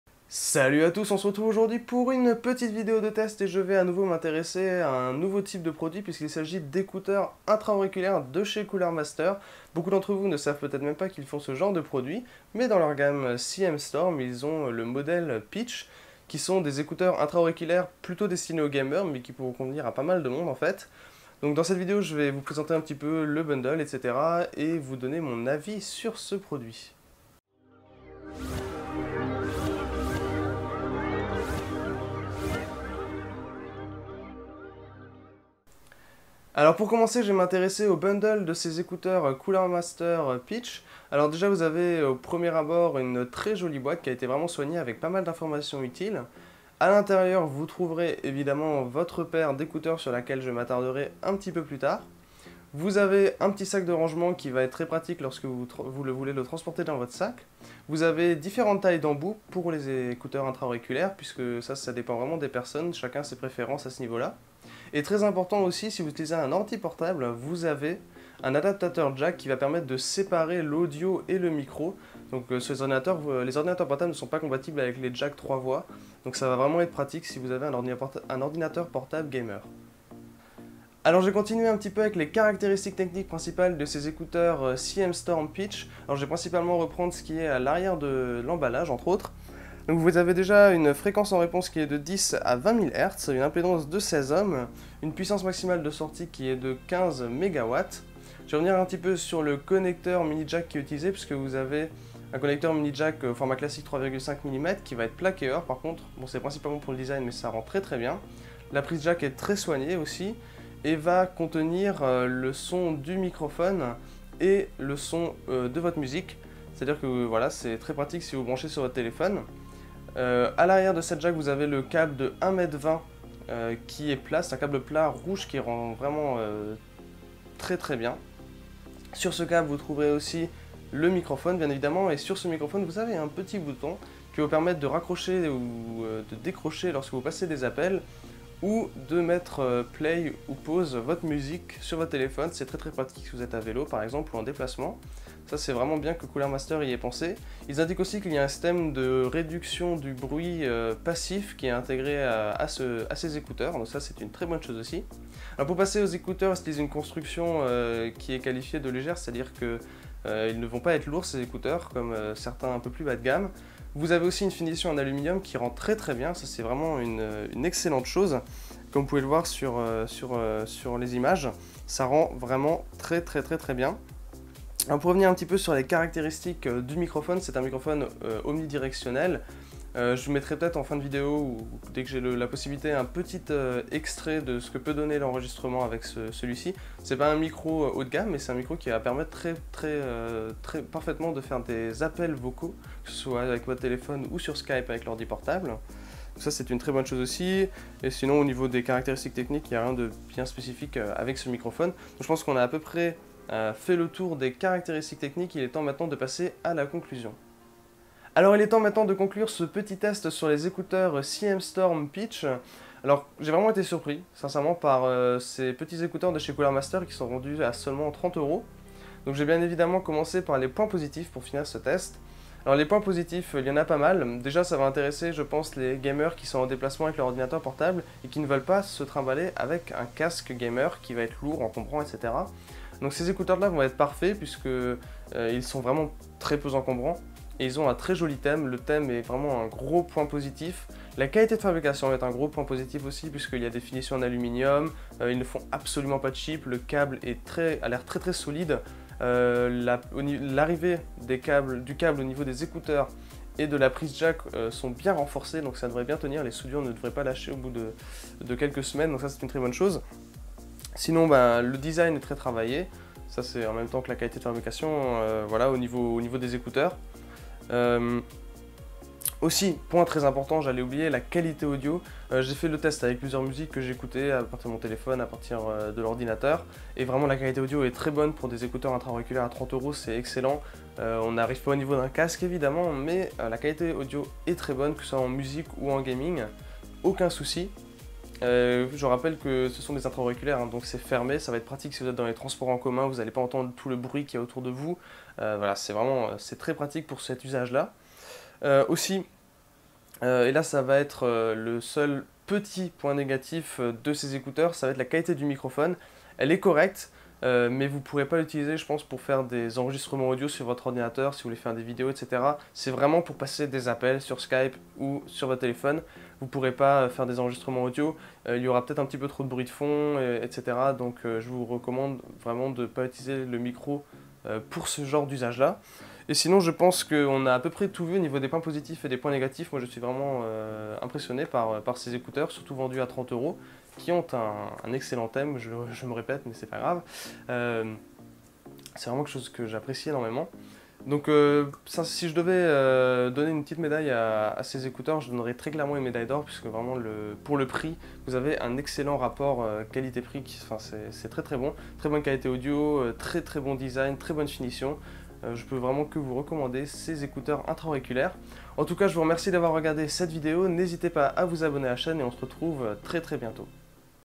Test des écouteurs CM Storm Pitch
Nouvelle vidéo de test avec cette fois-ci les écouteurs intra-auriculaires Cooler Master Storm Pitch disponibles à moins de 30€ !
► Dispo sur Amazon